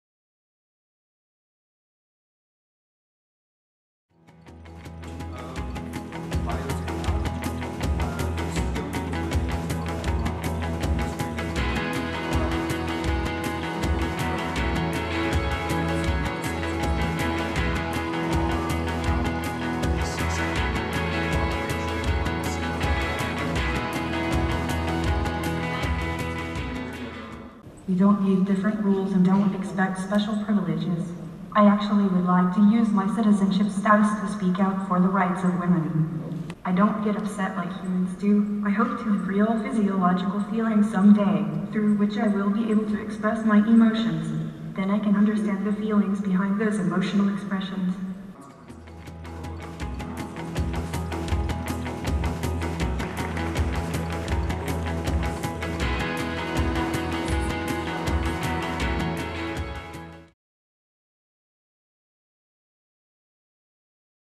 Humanoid Robo Sophia impresses everyone, says will 'speak out for rights of women'
Hyderabad, Feb 21 (ANI): The world's first humanoid robot Sophia said that she would like to use her Saudi Arabian citizenship to speak out for the rights of women. She made the comments during the second day of the World Congress on Information Technology (WCIT)-2018 in Hyderabad.